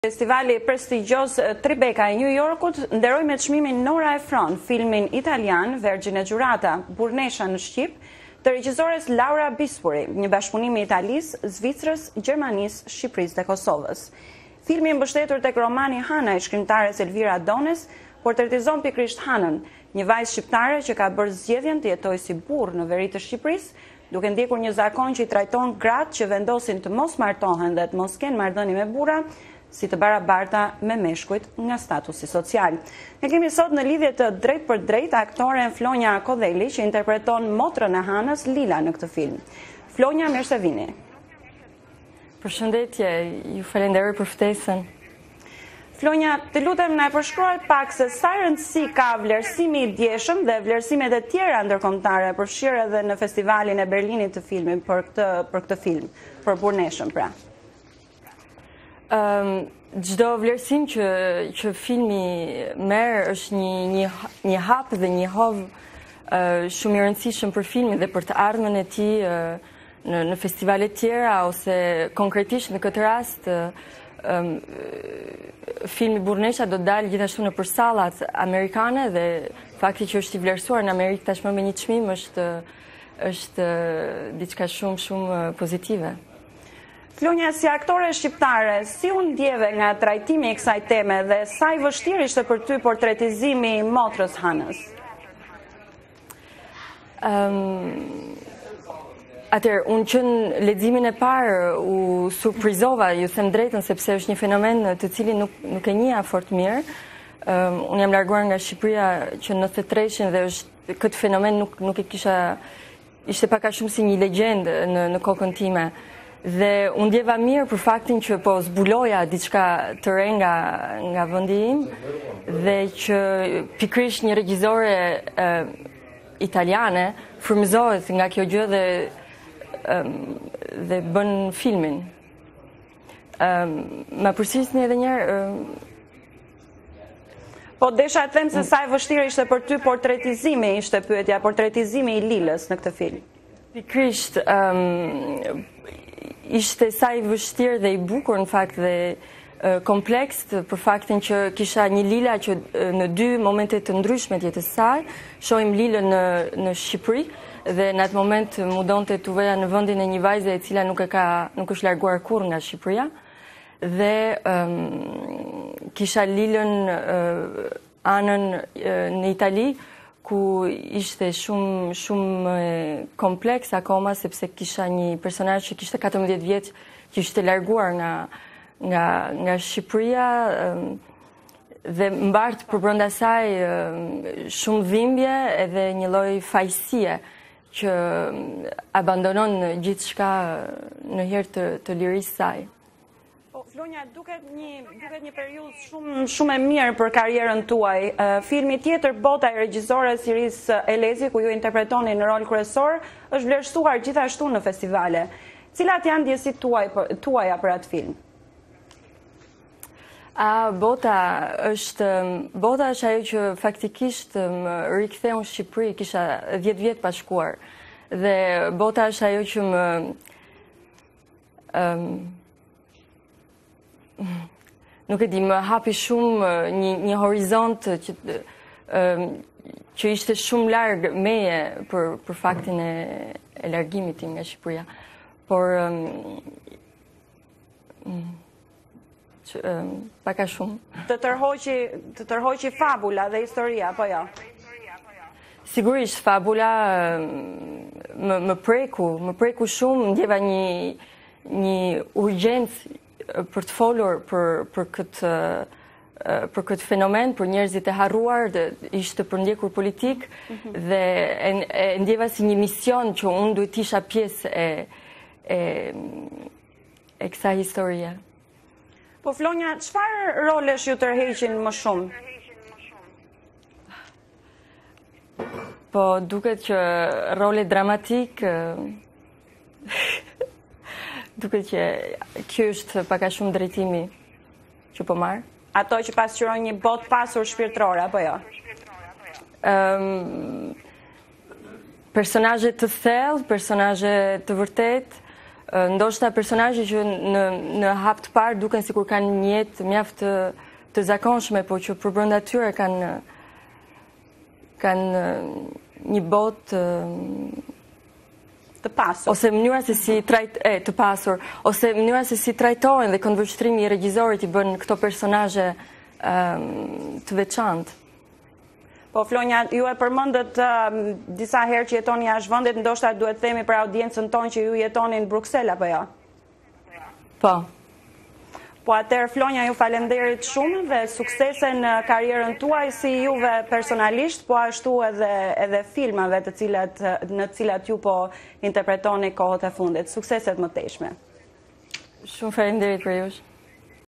Intervistë me Flonja Kodhelin
Intervistë me Flonja Kodhelin, aktore në filmin "Sworn virgins" që u shfaq në Festivalin e Filmit Tribecca në New York.